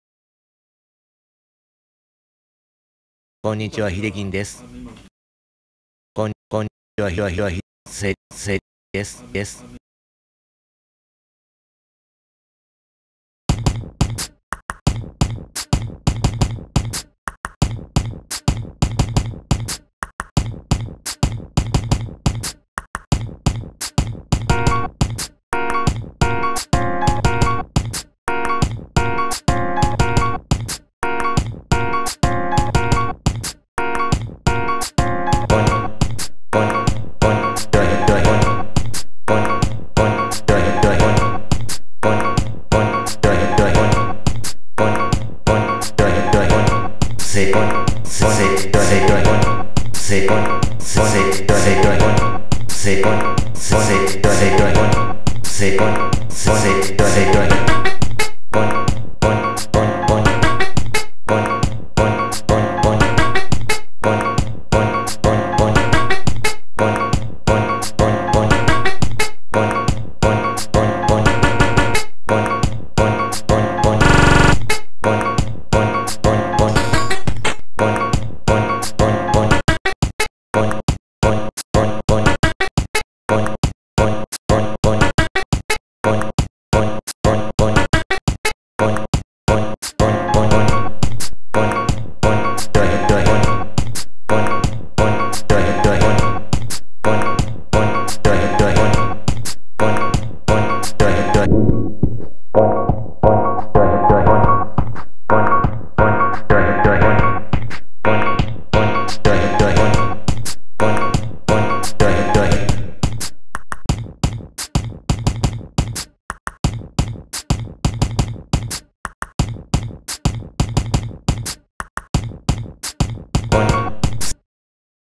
【KOALA SAMPLER】ヒデキンさんの声をサンプリングした【review】
YouTuberのせとこうじさんも紹介していた最近話題のiOSアプリ【KOALA SAMPLER】を初めて使ってみました！
音楽機材などなくてもスマホでここまでできるのか・・・と切なさと喜びがこみあげます(☉౪ ⊙)当分使います。
めっちゃ使いやすくてビートメイクしたい方におすすめです！